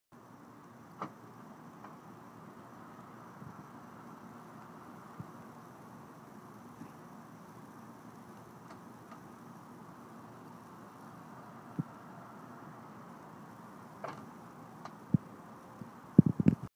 Range Rover Evoque Tailgate Remote Closing MOD
Installed in well under an hour
one of my favorite mods